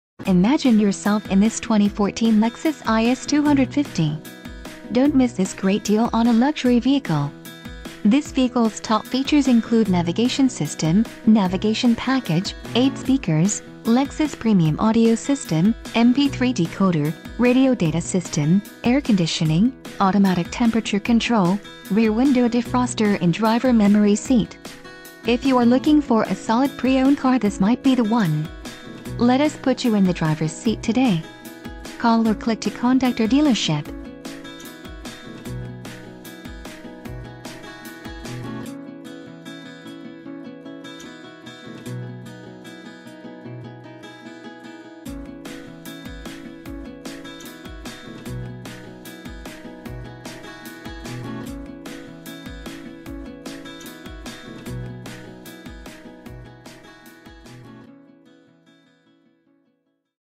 2014 Lexus IS 250 - Autobahn USA - Westborough , MA 01581
2014 Lexus IS 250.  STILL UNDER MANUFACTURER'S ORIGINAL WARRANTY, FULLY SERVICED AND FRONT LINE READY, NAVIGATION SYSTEM, BACK UP CAMERA, POWER MOONROOF, BLIND SPOT MONITOR, LEXUS ENFORM, LEATHER SEATS, POWER FRONT SEATS WITH DRIVERS SEAT MEMORY, 3-STEP HEATED FRONT SEATS, 3-STEP VENTILATED FRONT SEATS, SATELLITE RADIO, BLUETOOTH CONNECTION, PADDLE SHIFTERS, HID HEADLIGHTS, LED HEADLAMPS, PUSH BUTTON START, and ALL WHEEL DRIVE.  There are used cars, and then there are cars like this well-taken care of 2014 Lexus IS.  This luxury vehicle has it all, from a posh interior to a wealth of terrific amenities.  If you want a ride with style, this is it. This wonderful Lexus is one of the most sought after used vehicles on the market because it NEVER lets owners down. At AUTOBAHN USA all our vehicles go through an extensive 128 point inspection, are fully reconditioned and detailed to perfection.